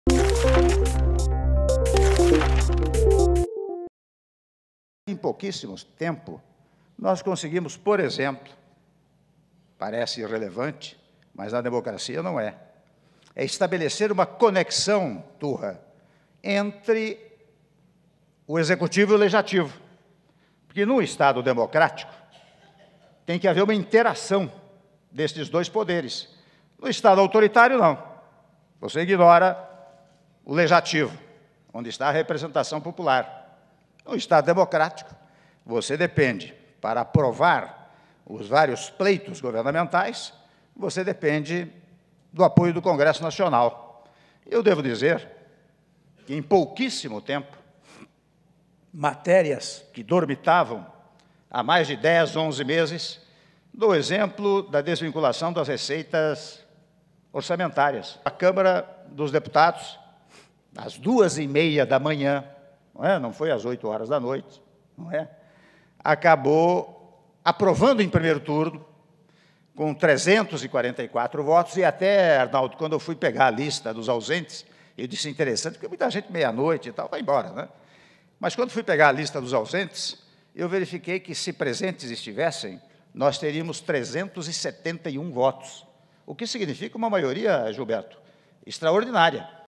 Presidente em exercício fala sobre a importância da relação do Executivo com o Legislativo
EM DESTAQUE  - 04.07.16: Durante abertura do maior evento de agronegócio do mundo, o Global Agribusiness Forum 2016 (GAF 16), o presidente em exercício, Michel Temer, falou sobre a importância do apoio do Congresso Nacional na aprovação de matérias essenciais para a Nação. Temer lembrou que em pouco tempo, seu governo conseguiu aprovar projetos que estavam parados há meses nas Casas Legislativas.